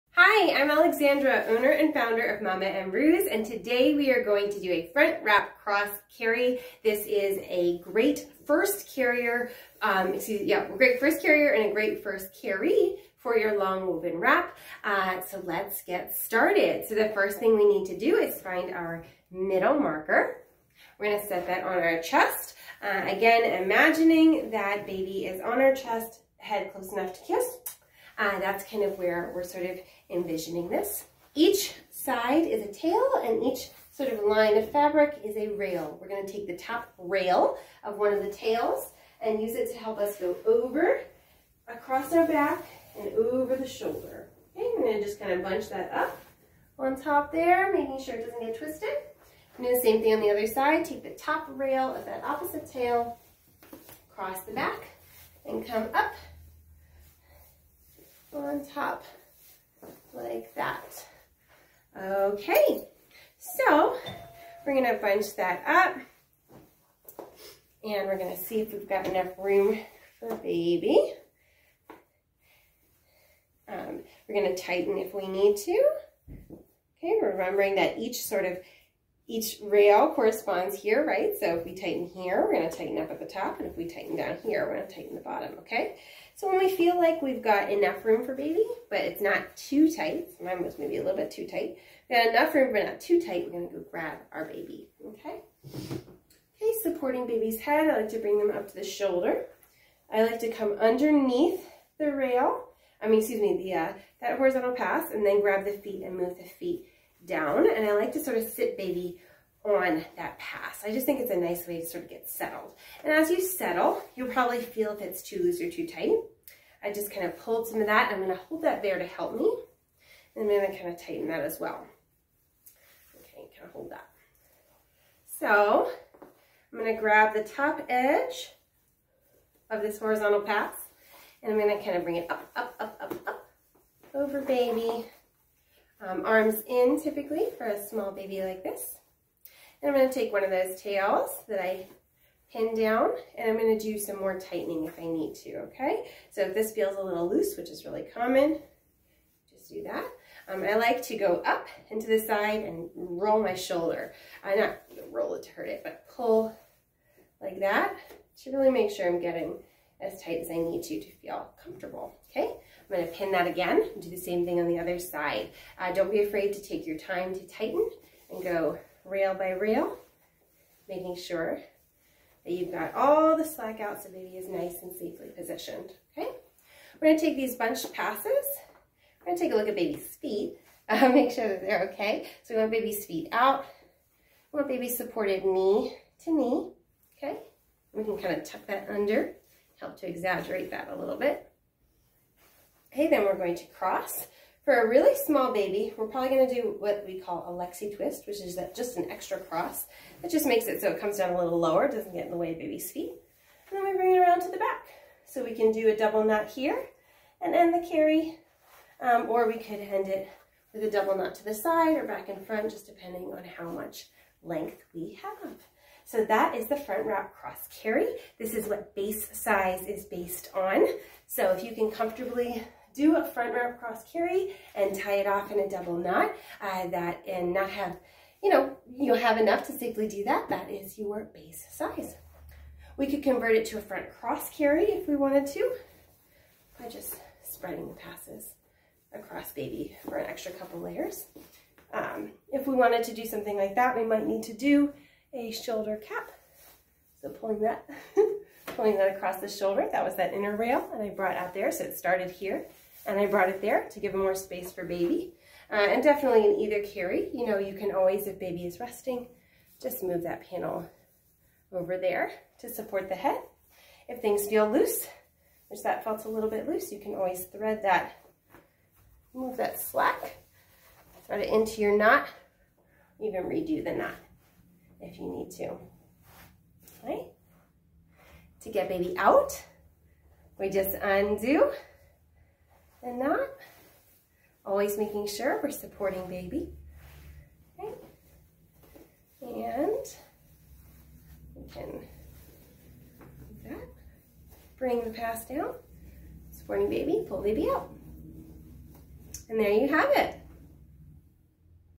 How to Front Wrap Cross Carry in a long woven wrap: The first carry you should learn
Join Mama & Roo's founder & babywearing educator in learning your first long woven wrap carry-- Front Wrap Cross Carry. "Base size" is whatever length you can comfortably wrap this carry in. Let's get started!

-First, find the middle marker and center the wrap on your chest. You want to imagine baby's head being close enough to kiss and adjust the height on your chest to the appropriate height

-Start with one of the tails (ends) of the wrap and go under your arm and up across your back to the opposite shoulder. Bring the tail over that shoulder. Repeat with the other tail across to the other shoulder

-You should now have 2 tails hanging in front of you. If you needed to, you could tie this off as a "pre-set" carry waiting for your little one to be ready to go in

-When you're ready, lift your baby up to your shoulder. Supporting your baby's head and neck the whole time, lower your baby into the pouch you've made with the first chest pass. You want this to be nice and tight around baby to give good tension. Lift the fabric up to your baby's neck (necessary for a tiny baby), and spread the lower rail (the bottom of the chest pass) from knee to knee. A smaller baby won't spread their knees as far as a bigger baby, but they will have more of a C-curve against your body than a larger baby. Finally, bring the bottom rail up in-between you and baby's body - up to about the diaper line. Adjust baby's position again - you want the baby's bum slightly lower than the knees, with the pass supporting baby knee to knee without making their legs stick straight out (so they can bend their legs freely) and they are comfortably curled against your body

-Now it's time to tighten! Pin one strap between your knees to hold tension. Lift the other strap up diagonally into the air and give a wiggle to start tightening, then go rail by rail (imagining the lines of the weave on a loom as stripes in your wrap that you are tightening one by one). Rail by rail, using your shoulder to help you move fabric as well, tighten the carrier. Remember that the top rail of the chest pass is equal to the top rail in the wrap tail/strap you are working with, so if you see slack there, make sure to tighten there in the tail/ strap. When you feel it's been sufficiently tightened, pin that strap and repeat the process with the other tail

-When you feel confident that it's been tightened sufficiently, it's time to tie off! For a little baby, you may want to add a Lexi Twist so that you aren't squishing baby legs. Bring the straps on either side of baby, twist (or not) at the base under baby's bum, and then tuck the tails under baby's legs and bring the tails to the back. Tie in a double knot, and you're done! =)

Looking for your own ethically made long woven wrap?